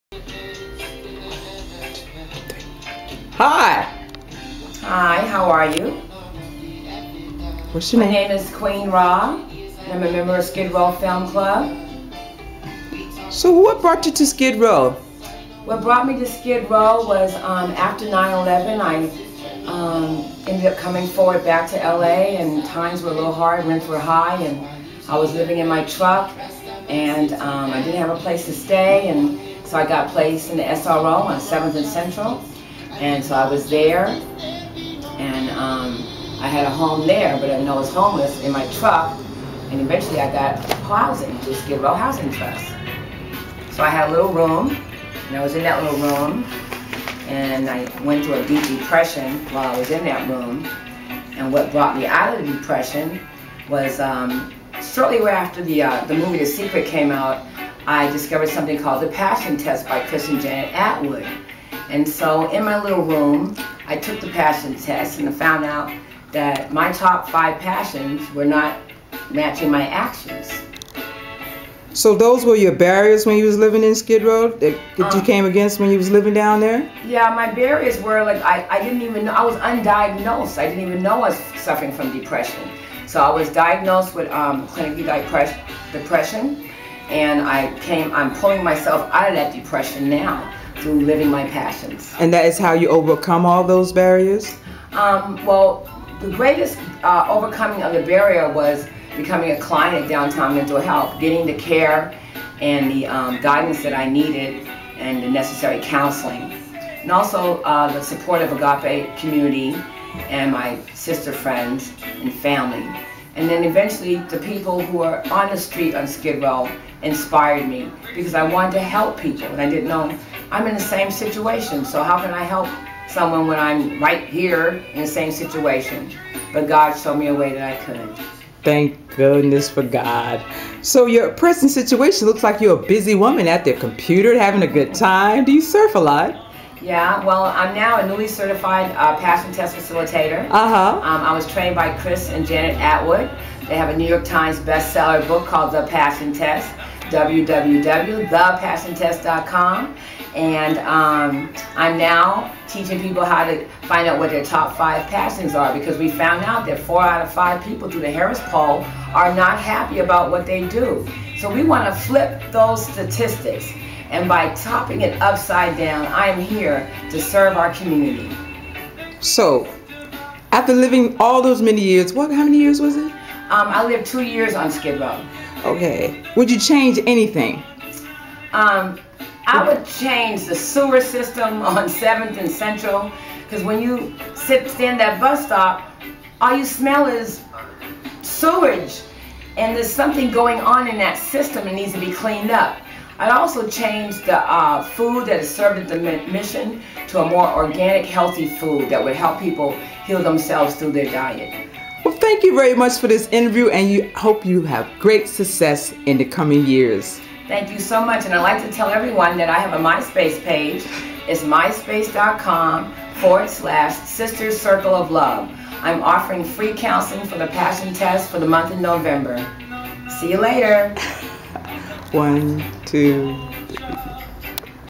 Queen at Sandra's
Voices of women in Skid row, Queen is very active these days. She's part of the reason voices has taken a new look. I hope everyone stays in tune with her adventurous life.